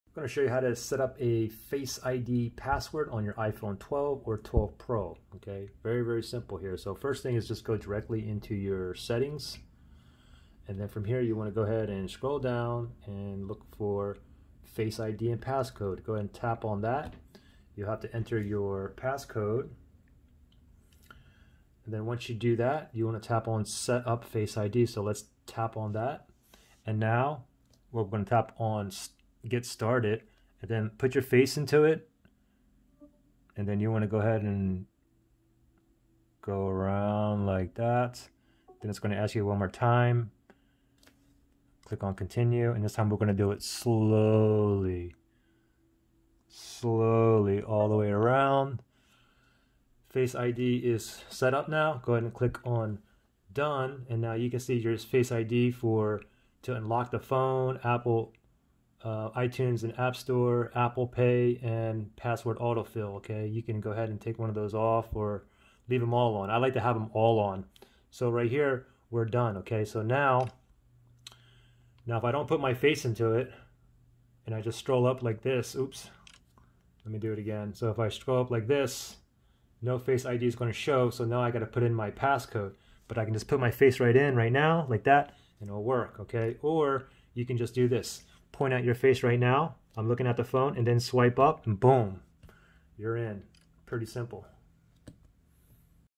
iPhone 12: How to Add/Setup Face ID Password
I show you how to add / setup a face ID password on the iPhone 12 or 12 Pro. Hope this helps.